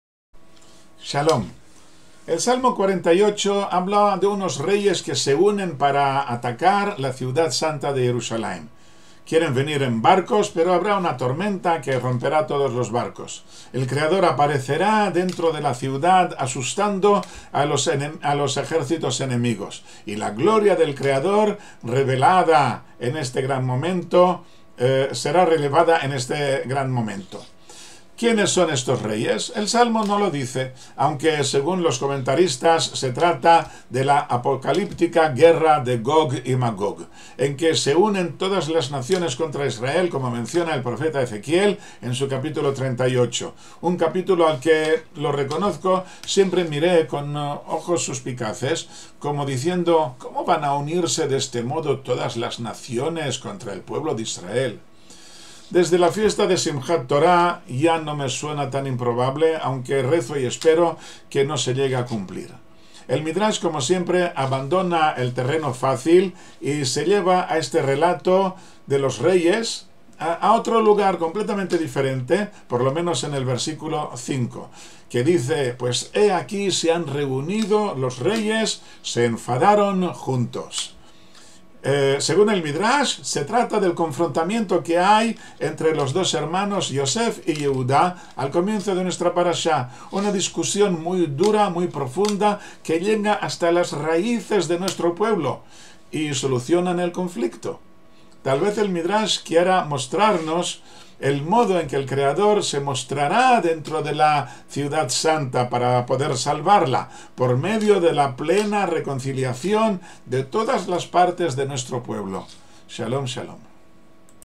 Se unen los reyes
Comentario a la parashá de Wayigash (Génesis 44:18 - 47:27)